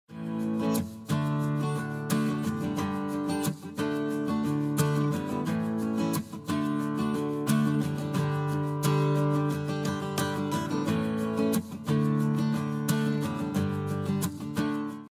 Intro templates for youtube ☕️ cooking ☕️ channel | No Copyright | 🍹 Free Download! 🍛
🍳 Kitchen intro for your youtube channel without text in HD template
🍝 You can use it for your youtube projects, but you can't re-upload it on your channel or make any top cooking videos, mention me in your video description.

I wish you a lot of success.
🍲Thank you very much for visiting me.🍲

🍜 I wish you all the success in everything you do.